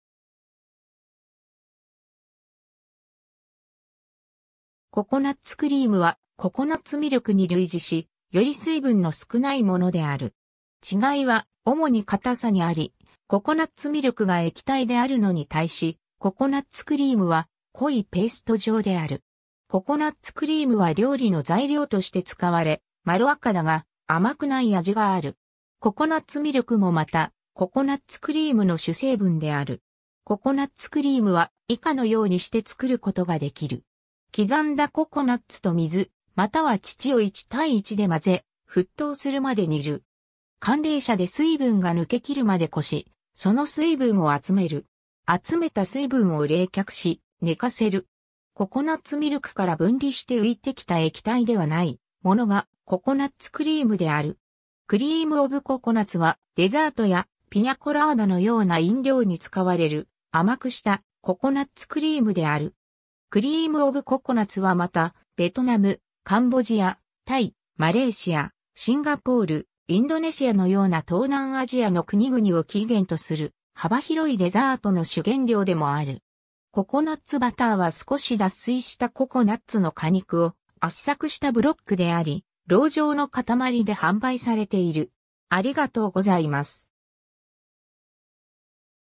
ココナッツクリーム
ココナッツクリーム(英語: Coconut cream)はココナッツミルクに類似し、より水分の少ないものである。
違いは主に硬さにあり、ココナッツミルクが液体であるのに対し、ココナッツクリームは濃いペースト状である。
ココナッツクリームは料理の材料として使われ、まろやかだが甘くない味がある。
 ココナッツミルクもまた、ココナッツクリームの主成分である。
 ココナッツクリームは以下のようにして作ることができる。
 刻んだココナッツと水または乳を1対1で混ぜ、沸騰するまで煮る。
 寒冷紗で水分(ココナッツミルク)が抜けきるまで越し、その水分を集める。
 集めた水分を冷却し、寝かせる。
 ココナッツミルクから分離して浮いてきた液体ではないものがココナッツクリームである。
 Cream of coconutはデザートやピニャ・コラーダのような飲料に使われる甘くしたココナッツクリームである。
Cream of coconutはまた、ベトナム、カンボジア、タイ、マレーシア、シンガポール、インドネシアのような東南アジアの国々を起源とする幅広いデザートの主原料でもある。
 ココナッツバターは少し脱水したココナッツの果肉を圧搾したブロックであり、蝋状の塊で販売されている。